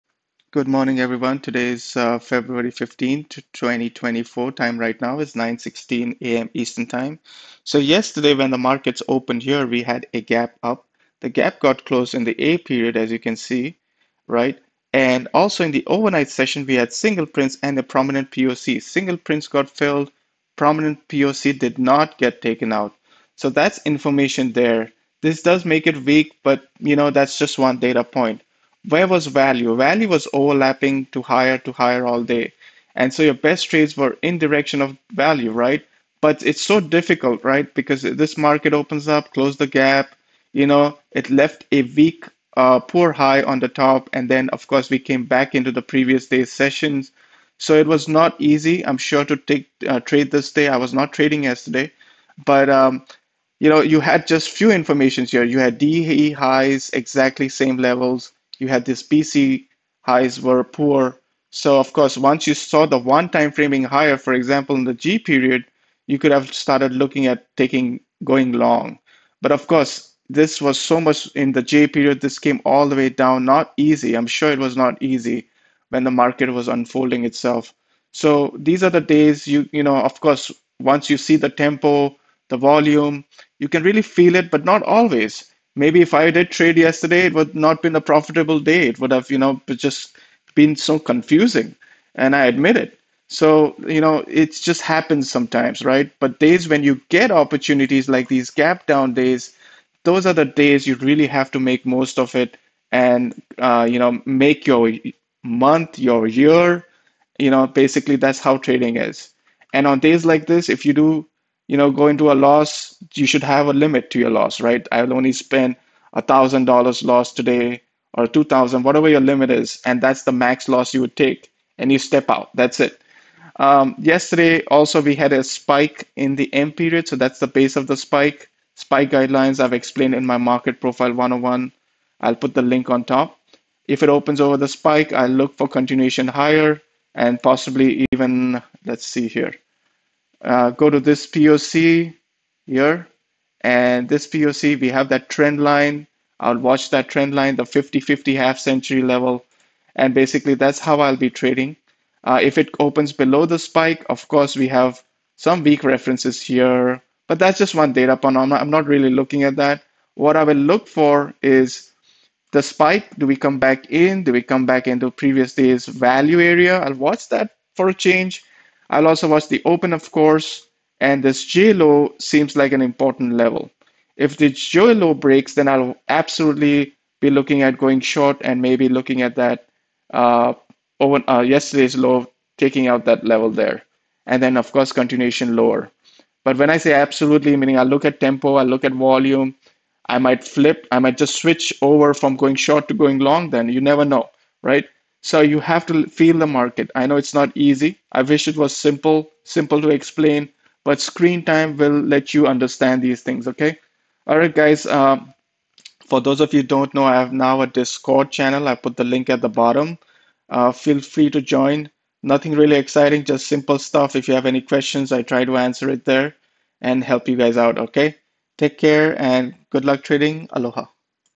Feb 15th: Pre-Market Analysis ES es_f spy: Spike, Value, Jan highs: Discord channel
(please watch all videos at 1.5x speed)

Spike guidelines in effect. Direction of value is important.